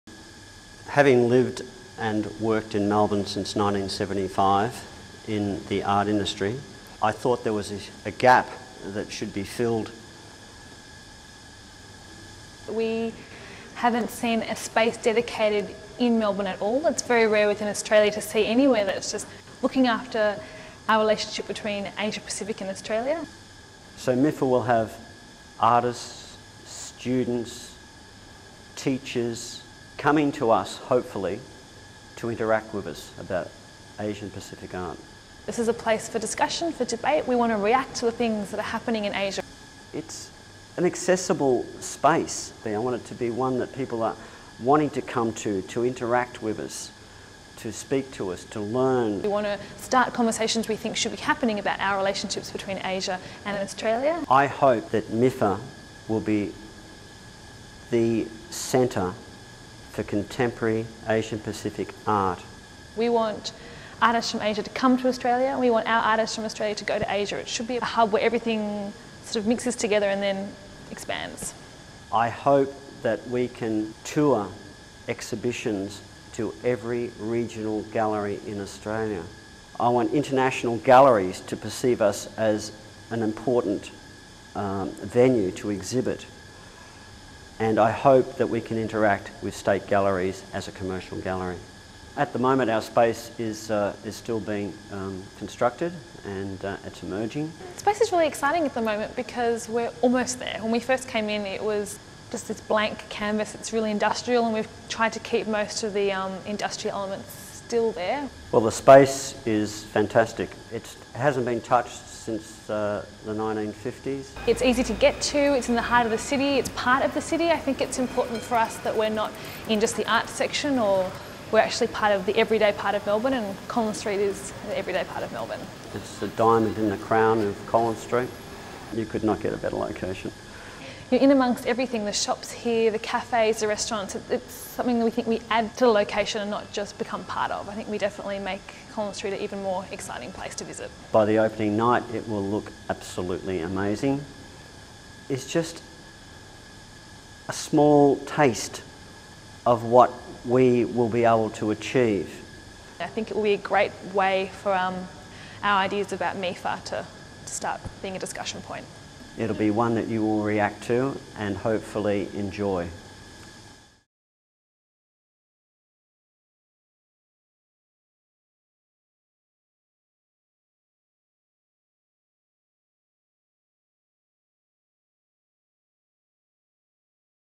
MiFA - A New Space
Hear what makes MiFA tick and what to expect from this new, exciting exhibition space.